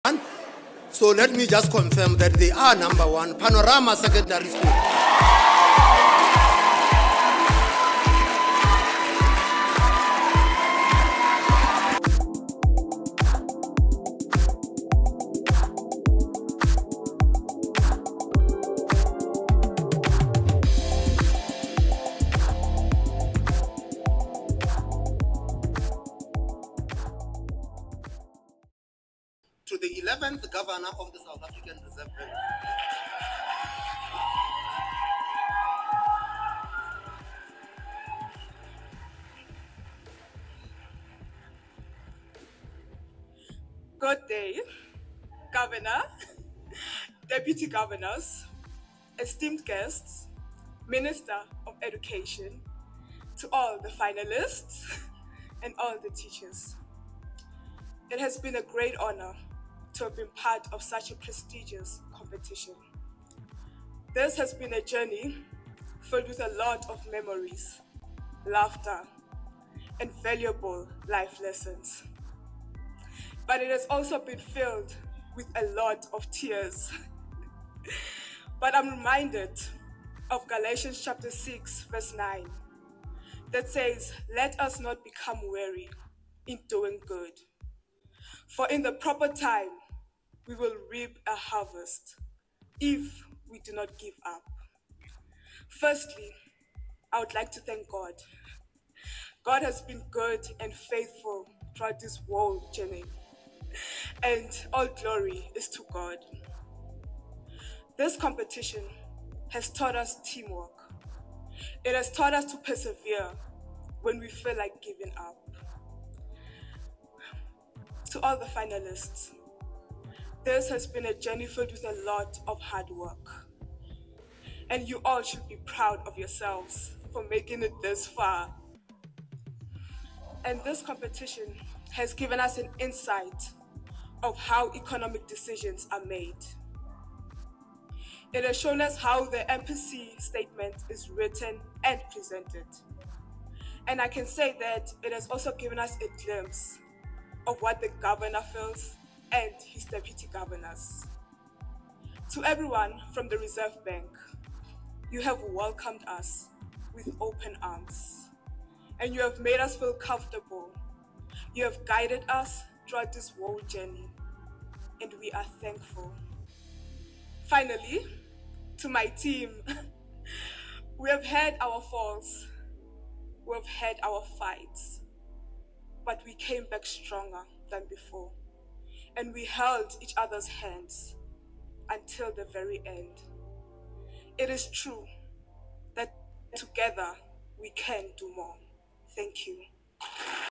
The Winners Of The “Monetary Policy Committee School Challenge 2024”
This was a school competition in economics. The even was help at Reserve Bank. The winners was the Paronama Secondary School. They won a R50 000+ and tablets each student and a laptop for the teacher.